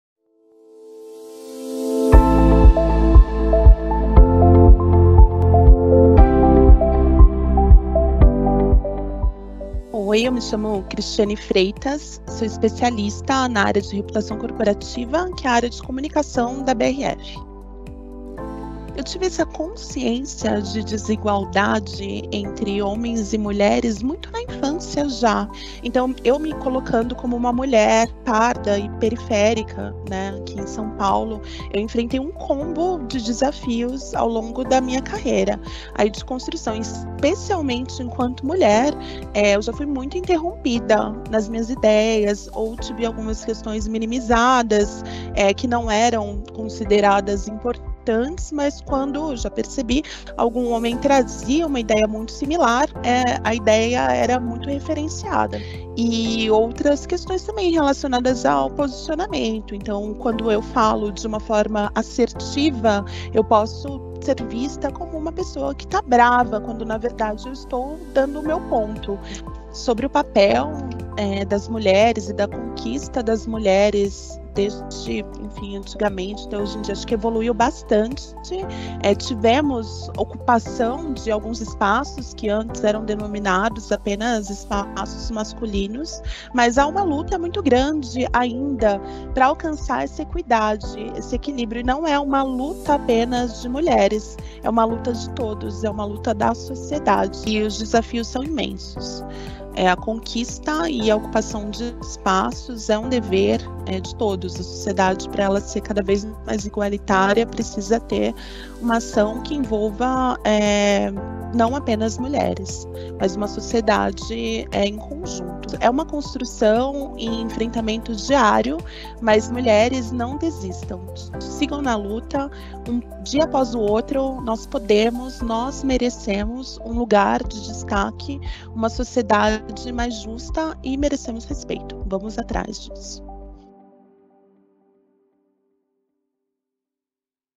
Dia Internacional das Mulheres – Cristiane M. Freitas
Neste mês, estamos homenageando as mulheres que ajudam a construir a BRF. Para fechar a semana, vamos apresentar a trajetória de mais uma colaboradora que, com muita dedicação, está conquistando seu espaço na companhia e no mundo. Conheça a história de Cristiane M. Freitas, nossa Especialista de Reputação Corporativa, que sempre batalhou pelos seus direitos, por um mundo mais igual.